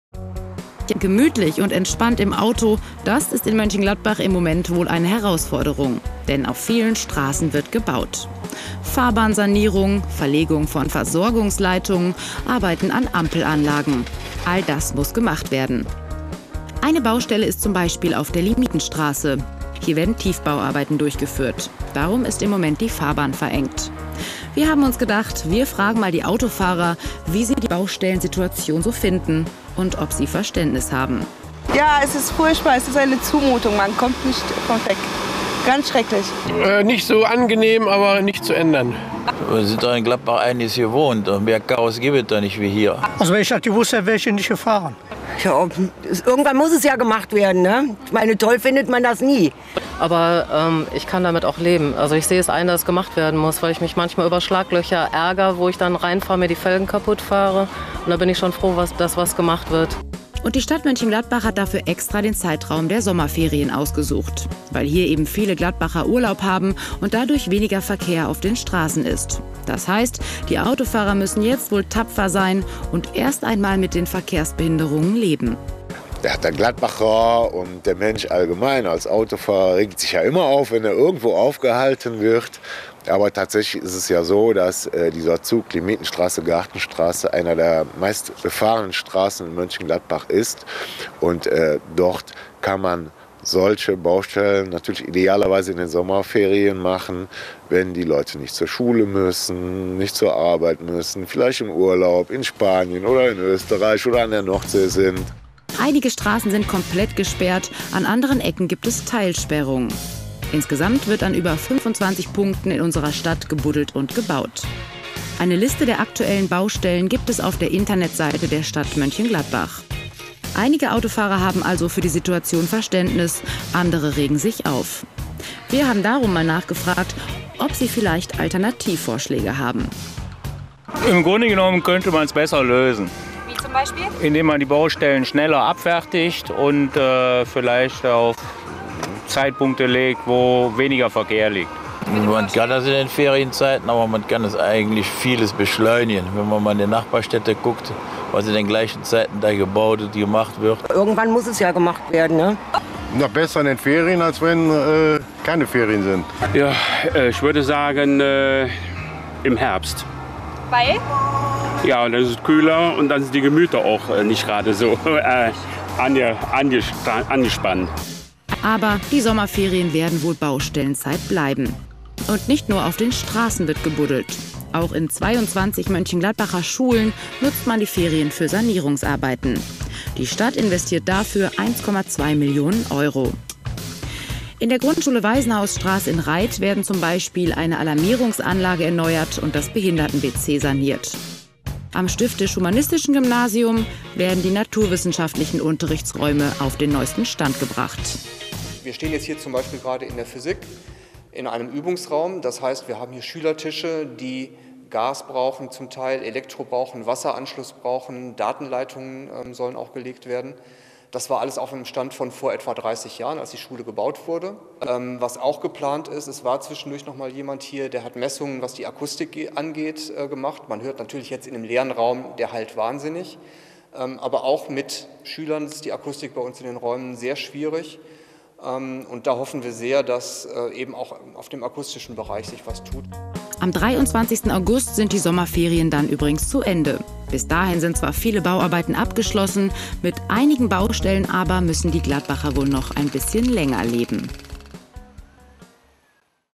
Ferienzeit ist Baustellenzeit
Auf den Straßen der Stadt wird gebaut und gebaut - und viele Autofahrer sind genervt... Aber: was sein muss, muss sein!